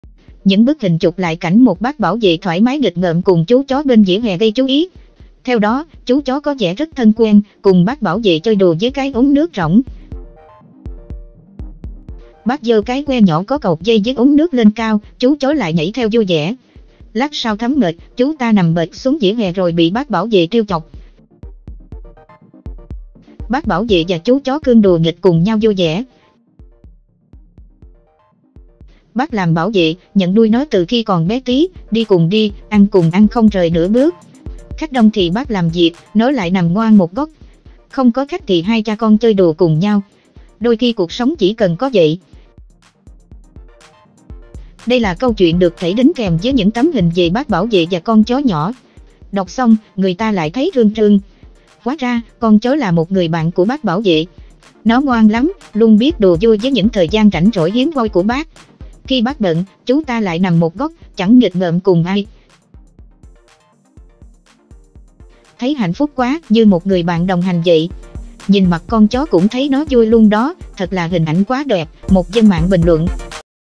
✅ Bác bảo vệ và hành động đùa giỡn cùng chú chó bên vỉa hè, câu chuyện đằng sau cảm động hơn
Đặt Mua  Qua ZALO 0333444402

✅Những bức hình chụp lại cảnh một bác bảo vệ thoải mái nghịch ngợm cùng chú chó bên vỉa hè gây chú ý. Theo đó, chú chó có vẻ rất thân quen, cùng bác bảo vệ chơi đùa với cái ống nước rỗng.
Bác giơ cái que nhỏ có cột dây với ống nước lên cao, chú chó lại nhảy theo vui vẻ. Lát sau thấm mệt, chú ta nằm bệt xuống vỉa hè rồi bị bác bảo vệ trêu chọc.
Bác bảo vệ và chú chó cưng đùa nghịch cùng nhau vui vẻ.
"Bác làm bảo vệ, nhận nuôi nó từ khi còn bé tí, đi cùng đi, ăn cùng ăn không rời nửa bước. Khách đông thì bác làm việc, nó lại nằm ngoan một góc. Không có khách thì hai cha con chơi đùa cùng nhau. Đôi khi cuộc sống chỉ cần có vậy".
Đây là câu chuyện được kể đính kèm với những tấm hình về bác bảo vệ và con chó nhỏ. Đọc xong, người ta lại thấy rưng rưng. Hóa ra, con chó là một người bạn của bác bảo vệ. Nó ngoan lắm, luôn biết đùa vui với những thời gian rảnh rỗi hiếm hoi của bác. Khi bác bận, chú ta lại nằm một góc, chẳng nghịch ngợm cùng ai.
"Thấy hạnh phúc quá, như một người bạn đồng hành vậy. Nhìn mặt con chó cũng thấy nó vui luôn đó, thật là hình ảnh quá đẹp", một dân mạng bình luận.